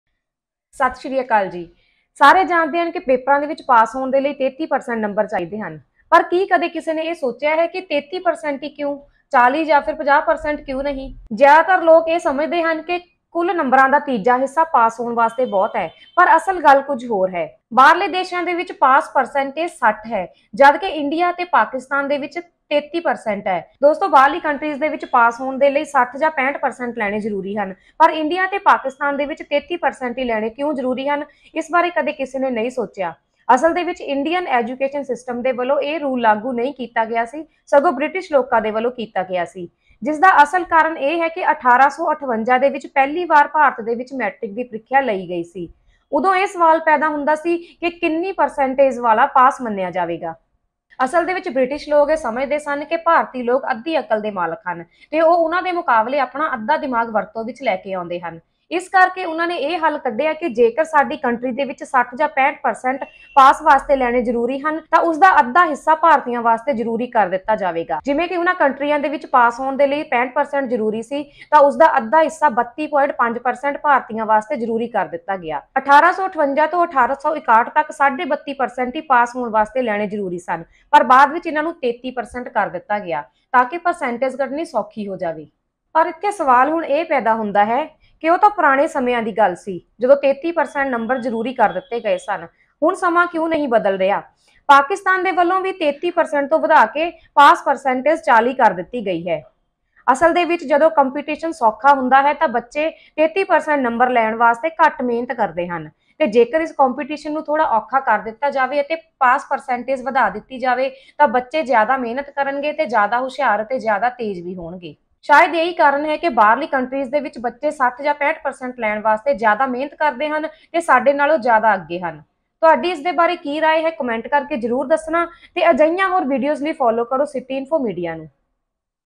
ਪਾਸ ਹੋਣ ਲਈ 33% ਨੰਬਰ ਹੀ ਕਿਉਂ ਜ਼ਰੂਰੀ | Why is only 33% marks necessary to pass?
And, get more latest videos of news, events and daily entertainment